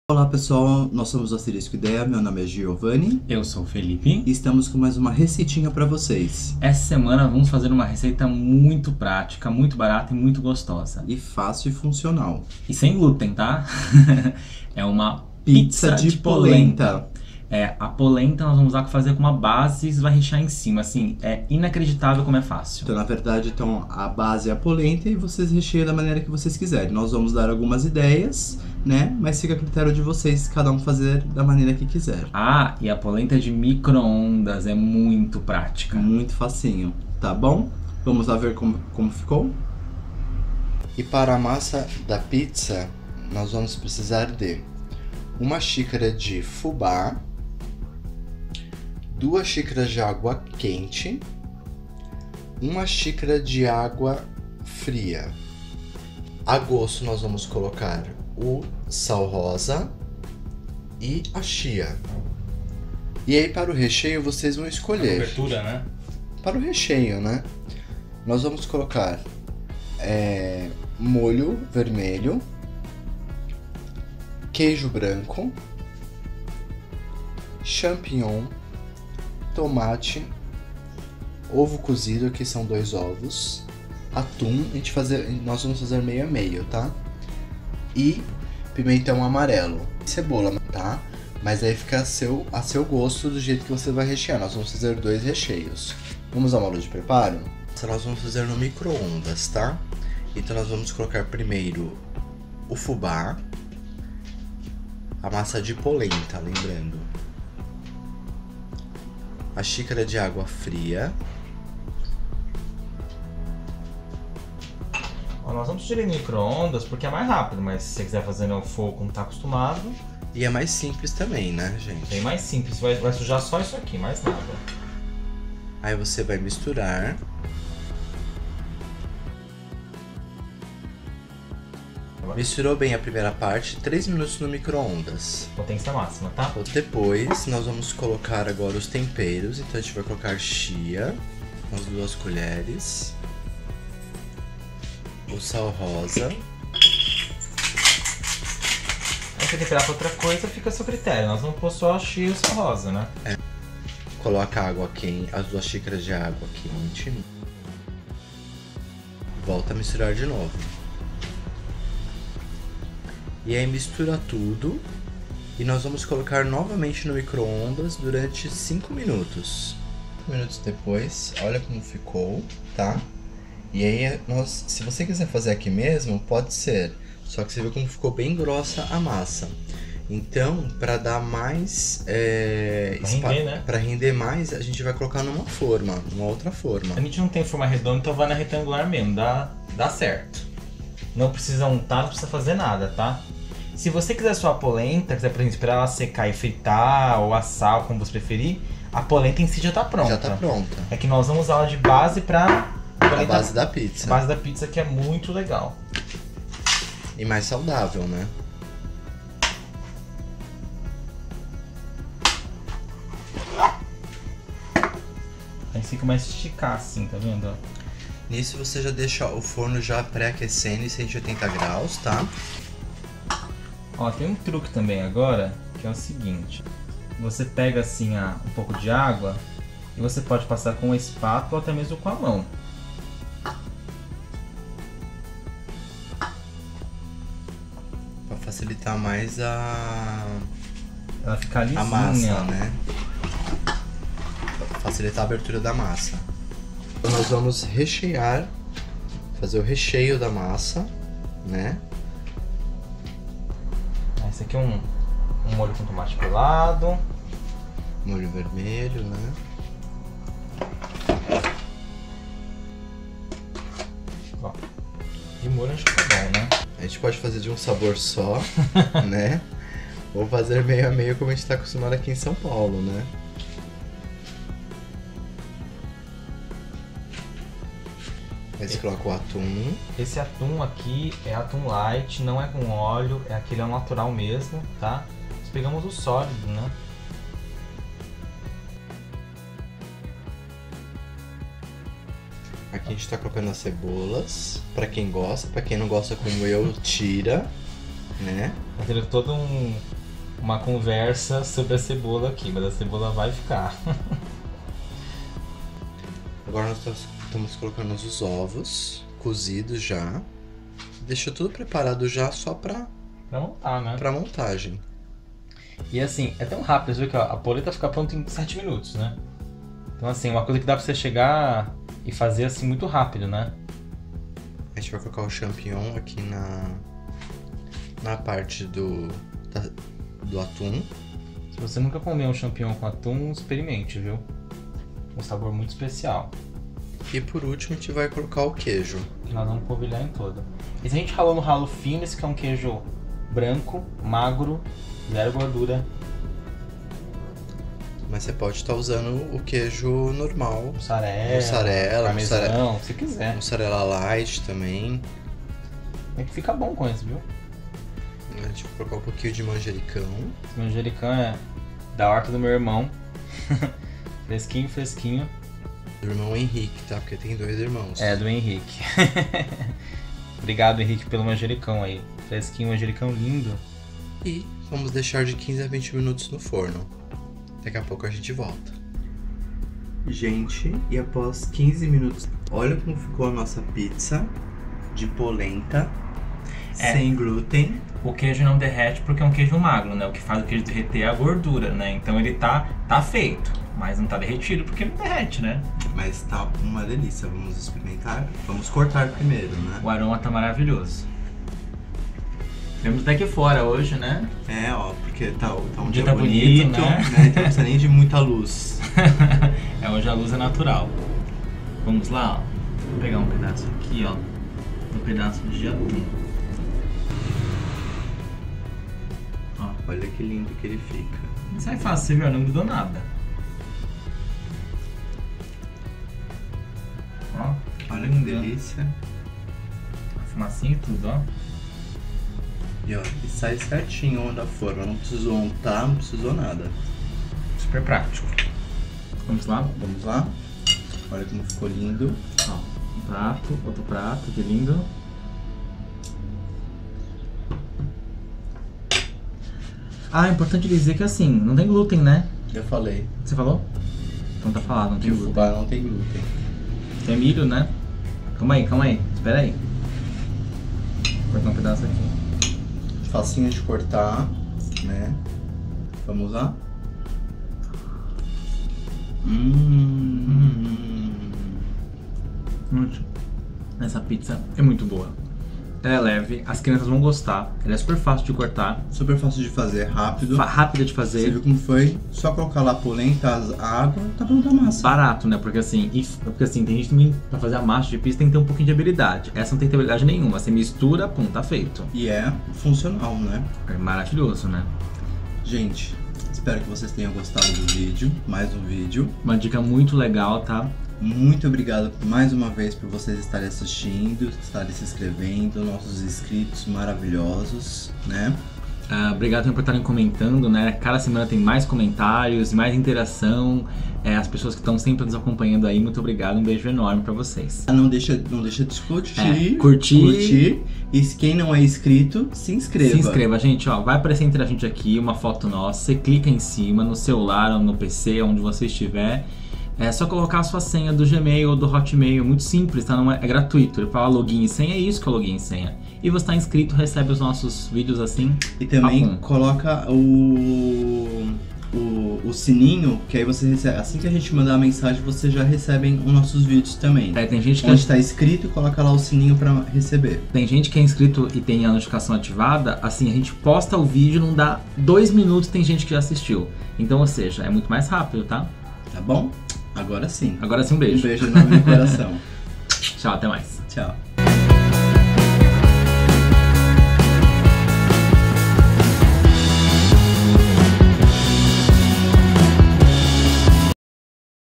PIZZA DE POLENTA Fit
Sim, estamos sugerindo uma pizza fit em que a massa é de polenta feita com fubá fino e base preparada no microondas! Tudo muito fácil, simples e bem gostoso!

Sugerimos aqui 2 opções de recheio para a pizza, mas a opção é sua do que prefere utilizar!

Deste modo, você faz sua polenta sem sujar panelas e prepara esta pizza bem rapidinho.

Já fizemos uma pizza doce de Melancia, como uma salada de frutas, confira aqui a receita

Segue abaixo a receita:
PIZZA FIT DE POLENTA

Acesse o nosso e veja a receita passo a passo como está descrita no vídeo!